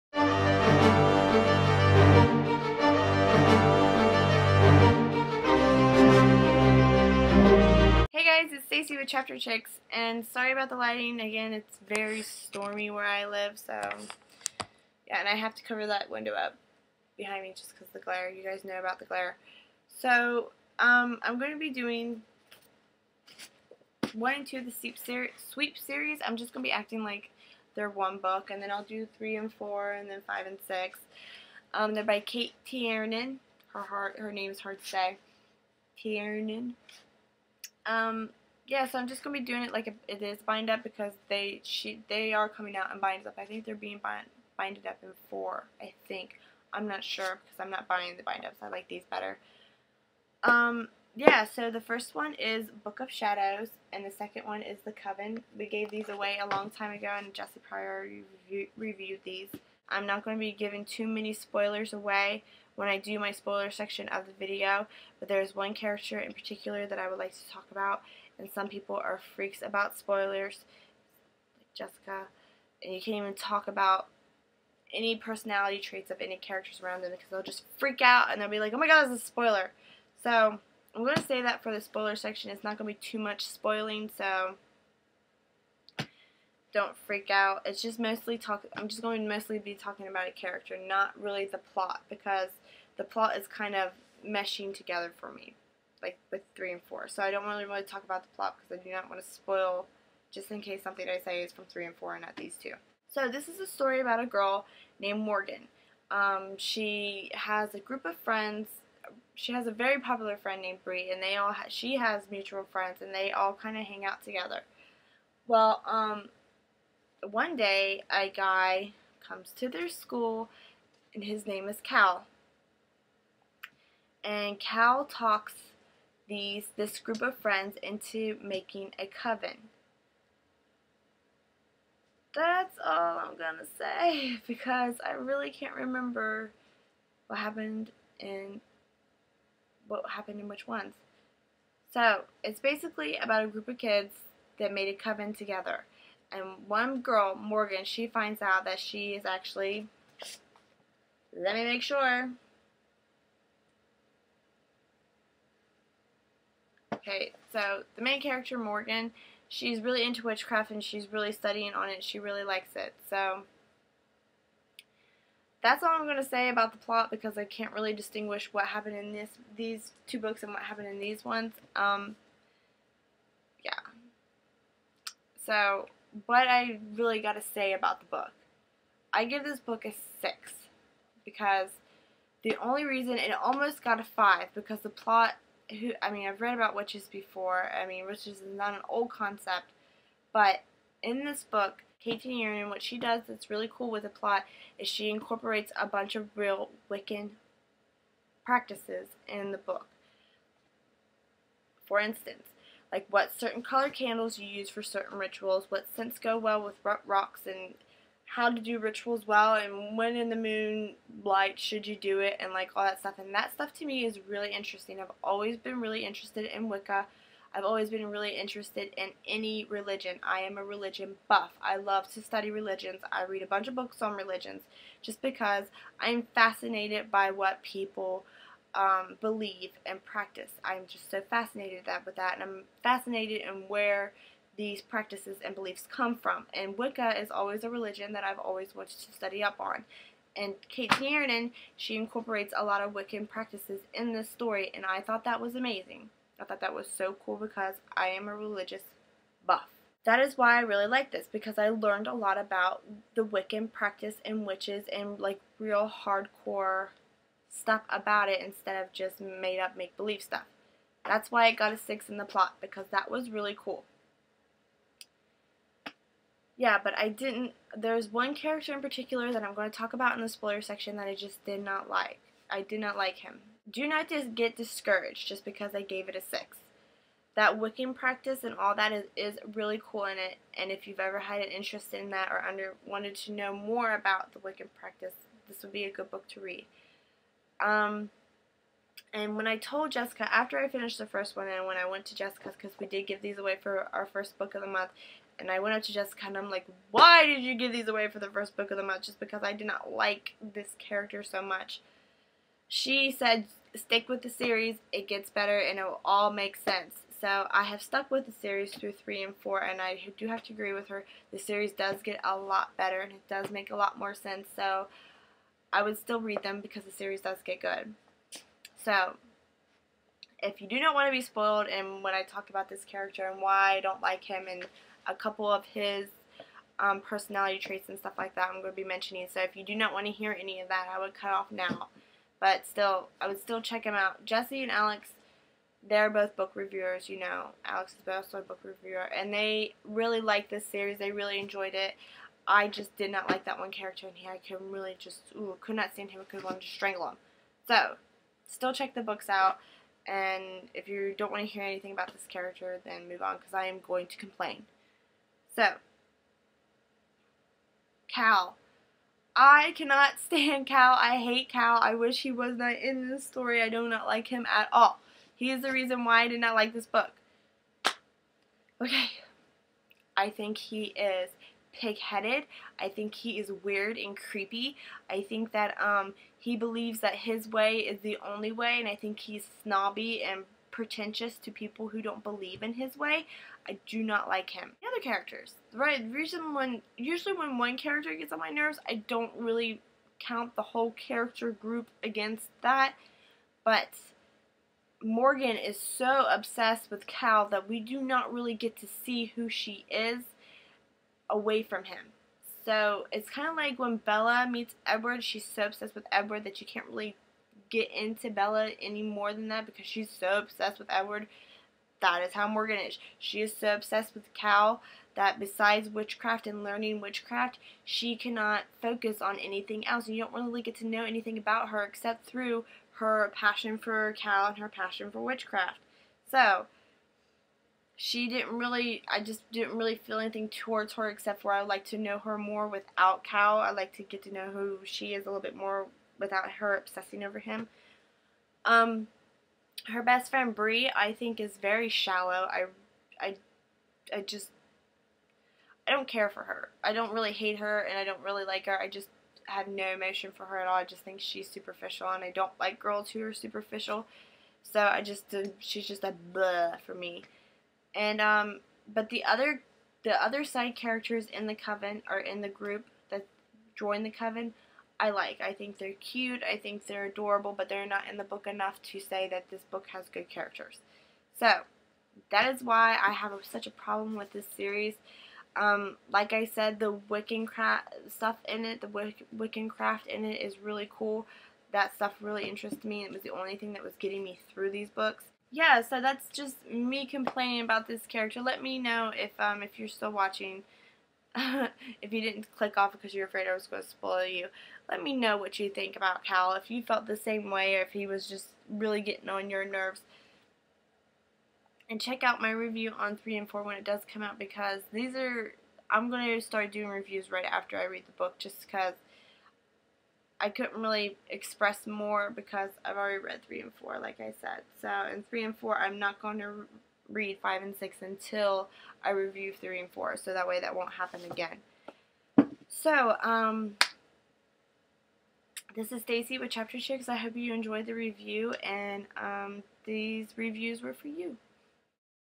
Sweep 1&2 Review
I am going to be doing the Sweep series reviews in sets of two. So here is the first and the second books in the Sweep series. I give these books 6 out of 10 stars. I hope you guys like the review.